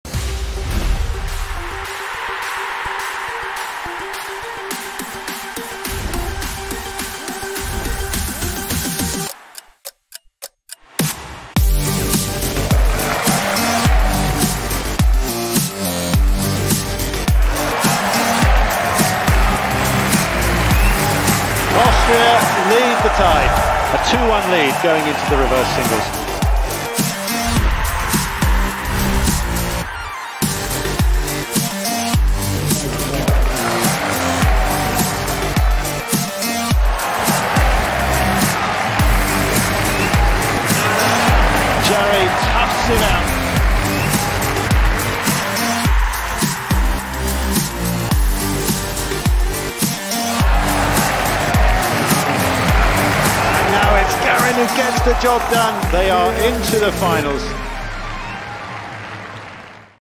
State of Play: Austria 2-3 Chile
Chile completed a thrilling comeback against Austria. Here's a recap of the second day's play in Salzburg.